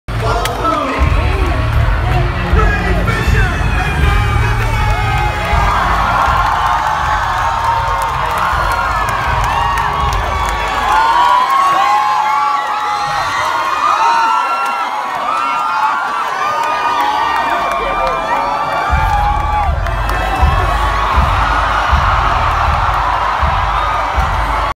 Gal Gadot Hall H Entrance Comic Con 2017
Gal Godoy try's to save a fanboy as he falls to her feet.